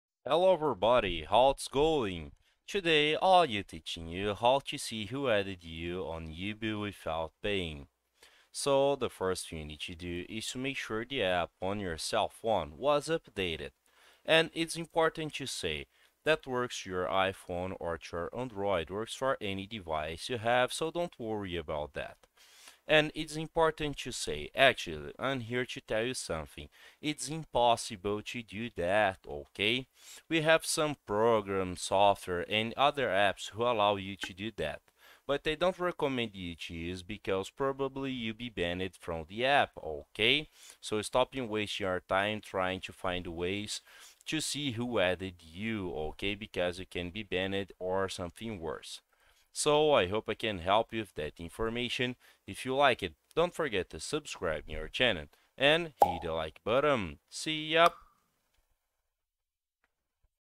How To See Who Added You on Yubo (2025) - FULL Guide
In this video I will show you how to see who added you on yubo without paying.

I hope I have helped you with the video!